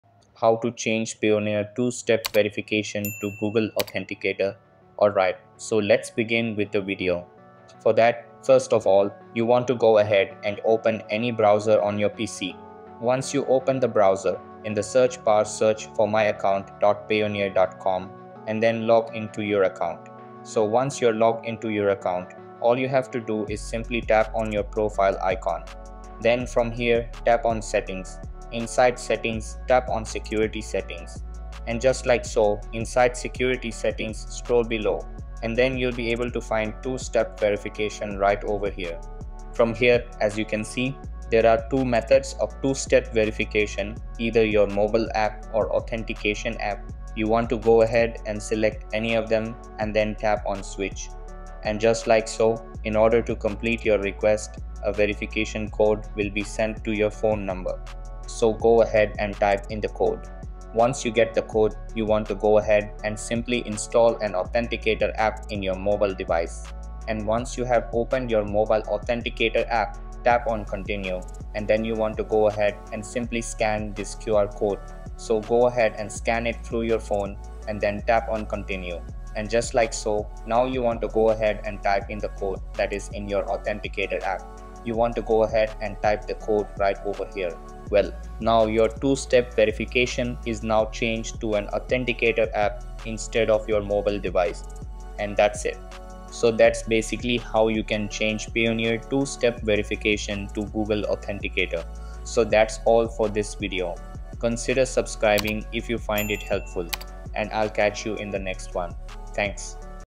How To Change Payoneer Two Step Verification To Google Authenticator
Change Payoneer Two Step Verification To Google Authenticator
How To Enable Two Step Verification Method In Payoneer Account